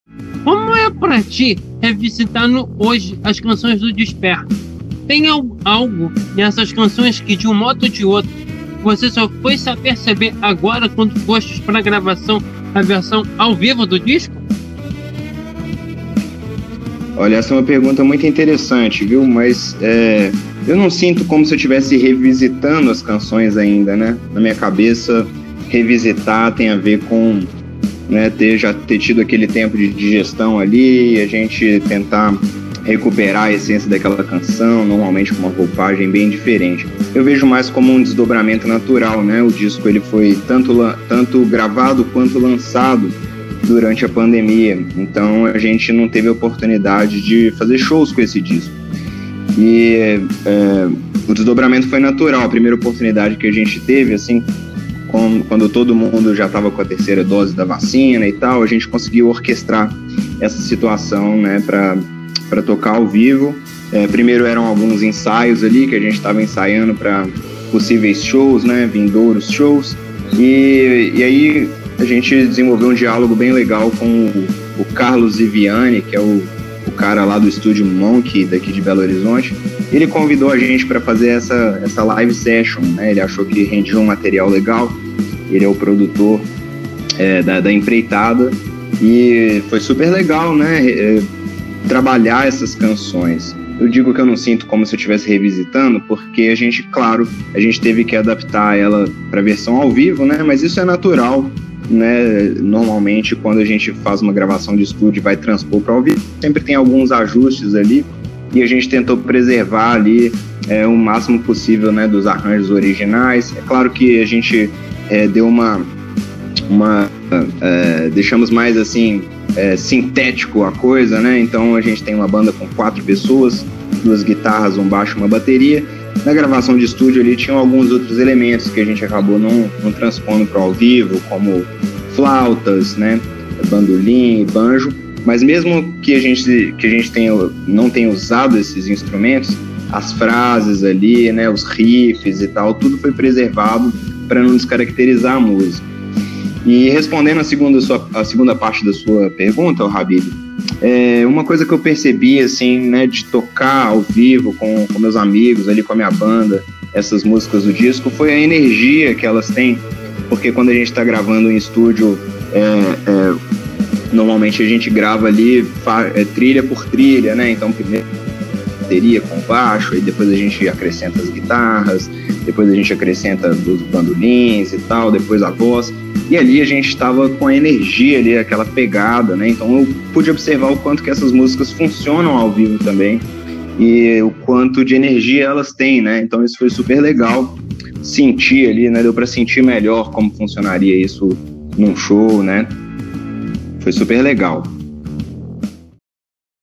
O DIZPERTAR DE UM NOVO SOM *** Vitrola
Salvem queridos e estimados vitroleiros tudo bem com todos ? Assim estimo que sim! Eis que hoje aqui em nosso canal temos  uma nova edição  do Vitrola Cortes, com trechos do melhor de nossas entrevistas que foram ao ar aqui em nosso canal !
Nosso corte de hoje, fica a cargo da Super entrevista que tivemos com o querido Fernando Mascarenhas lá de Minas Gerais !!! Quer conferir um cadinho de como foi essa prosa? Então tasca o play !!!
Tasca o Playzinho e confira um trecho de nosso super bate papo!